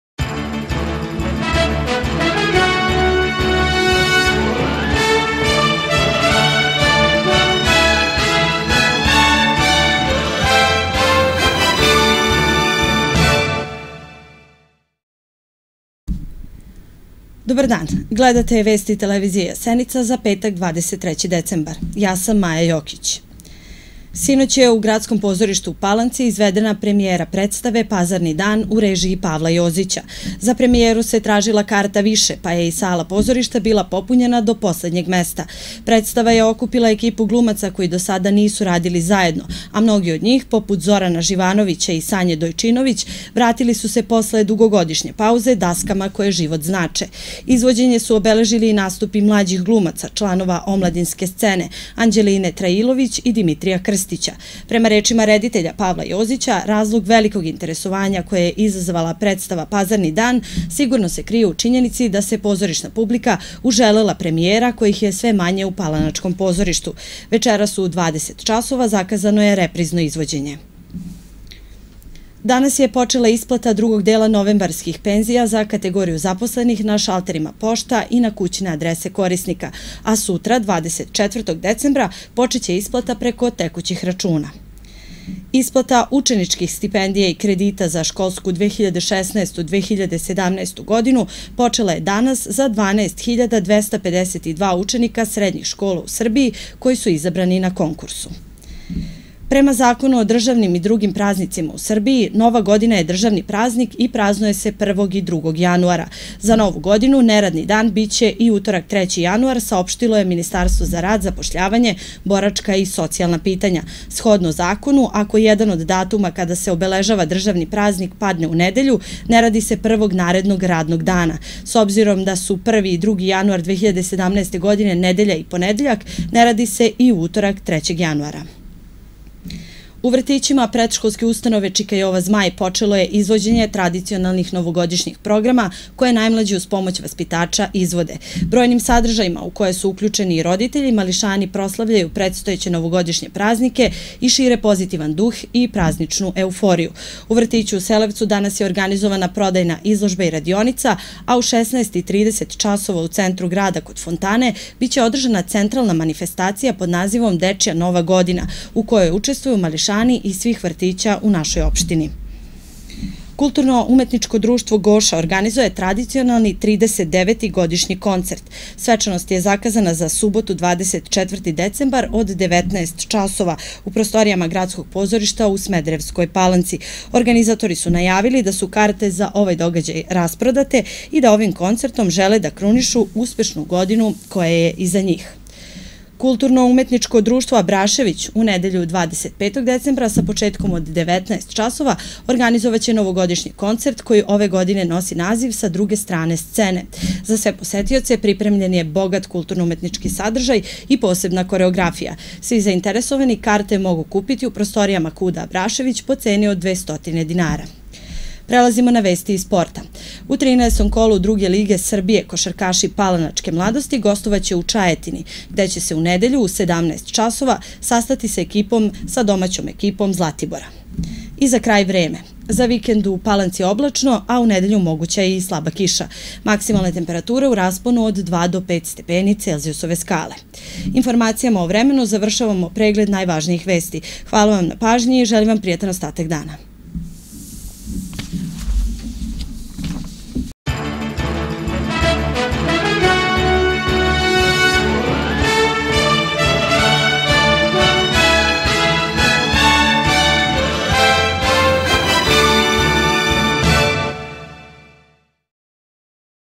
Vesti za 23. decembar 2016.
Emisija u kojoj možete saznati najvažnije vesti sa prostora Smederevske Palanke i okolnih opština, za svega nekoliko minuta.

Voditelji: Maja Jokić, Daša Stanošević, Svetlana Savić, Bojan Živković, Marko Ljubisavljević